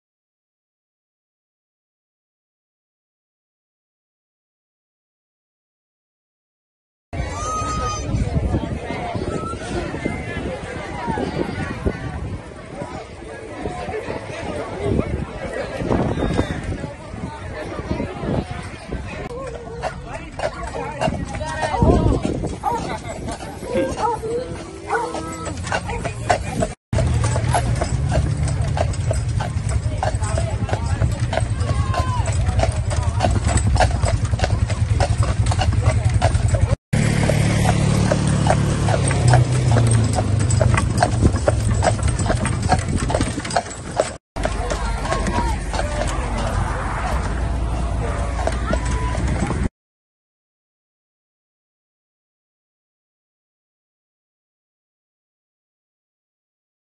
White Horse and Funeral Hearse Carriage Camden NJ with Bicycle Escort Carl Mller F H March 30 2021
White Horse and Funeral Carriage Hearse in Camden NJ escorted by bicycle riders. This was a funeral for one of those boys hit by a car. Carl Miller Funeral Home March 30 2021